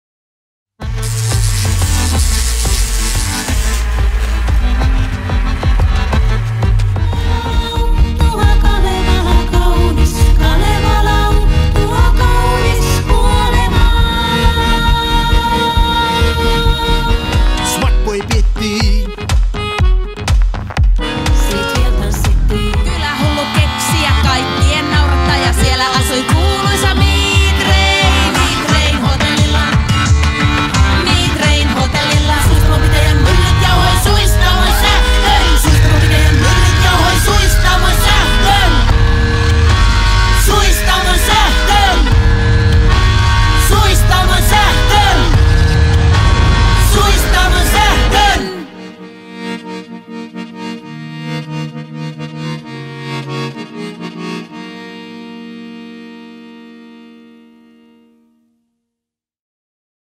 Suistamon Sähkö Trailer
Suistamo Electricity
This folktronica band stirs up hysteria and makes you dance yourself into a trance. Anne-Mari Kivimäki and Eero Grundström create new Suistamo dance music with electronics and a Notka accordion. The combination results in fascinating work by crazy scientists. Dancers Reetta-Kaisa Iles and Tuomas Juntunen bring to the stage the power of song, scintillating motion and manic moves. The songs' stories are inspired by, for instance, Suistamo's old village maps, hydroelectric power plants and mills, scratchy archive tapes, folk poetry, trips to Karelia, and bumpy roads. Suistamon Sähkö has something new, something old, something blue and something borrowed.

Suistamon Sähkö
Kansanelektrobändi lietsoo hysteriaa ja tanssittaa transsiin. Anne-Mari Kivimäki ja Eero Grundström tekevät Suistamon uutta tanssimusiikkia koneilla ja Notka-haitarilla. Yhdistelmä on hullujen tiedemiesten lumoavaa työtä. Lauluvoimaa, säkenöivää liikettä ja
maanista menoa lavalle tuovat esitanssijat Reetta-Kaisa Iles ja Tuomas Juntunen. Laulujen tarinoihin on haettu inspiraatiota muun muassa Suistamon vanhoista kyläkartoista, vesivoimalaitoksista ja myllyistä, rahisevista arkistonauhoista, kansanrunoudesta, Karjalan matkoista ja kuoppaisista teistä. Suistamon Sähkössä on jotain uutta, jotain vanhaa, jotain sinistä ja jotain lainattua.

Trailer by Kim Saarinen
suistamonsahko.fi